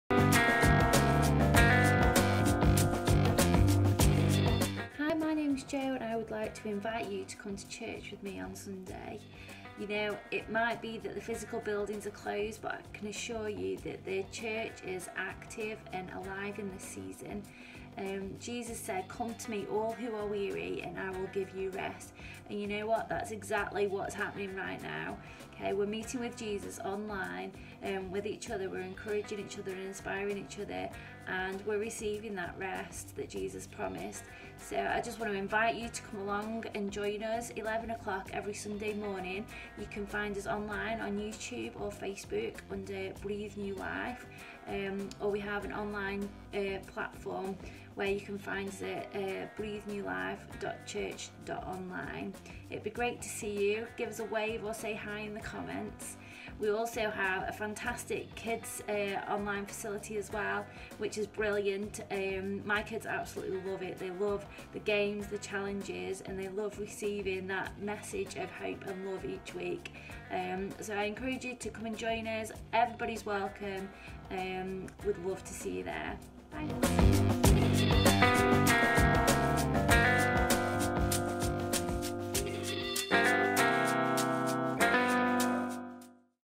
Your invite to Church Online from Jo
You are invited to join us for church online at 9:30am (Breathe Kids) and 11:00am every Sunday via the Church Online platform

You can find out more about Sundays at Breathe New Life via our

You can also join our mailing list by filling in this quick subscriber form